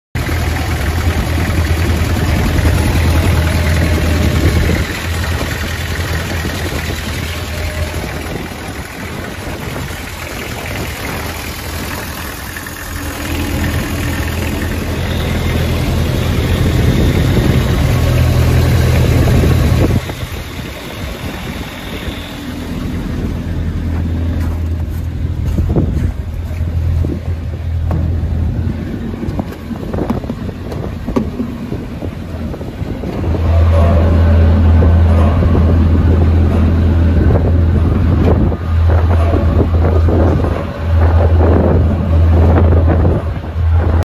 2007 FINN T90 For Sale
2007 FINN T90

- 419 hours
- Kubota diesel engine
- 250' of hose
- Functions great
- Comes with additional fittings and brass ends

- Retirement Sale